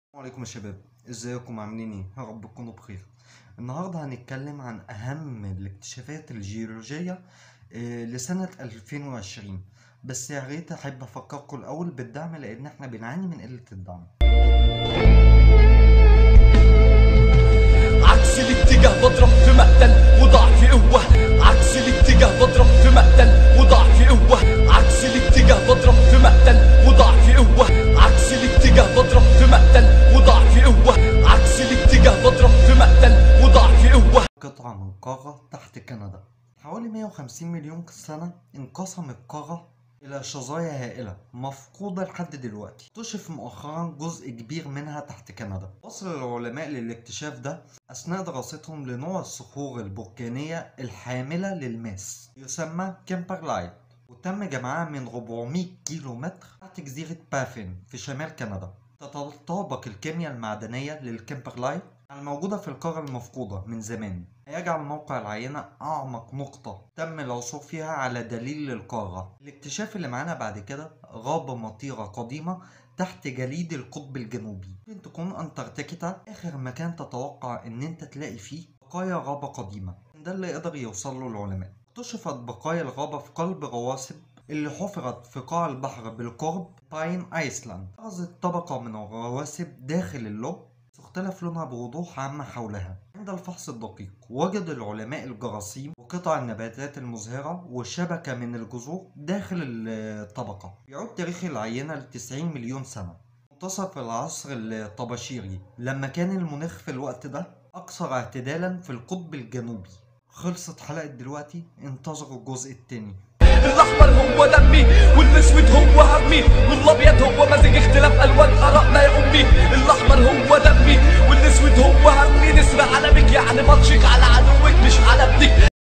اكتشاف قارة مفقودة + غابة قديمة
اكتشاف قطعة من قارة مفقودة تحت كندا
غابة مطيرة قديمة تحت جليد القطب الجنوبي
اكتشف العلماء هذا العام بعضا من أكثر أسرار الأرض روعة، حيث وجدوا أنهارا مخفية وقطعا من القارات المفقودة وبقايا غابات مطيرة قديمة، وغاصوا في التاريخ القديم باستخدام أحدث التقنيات.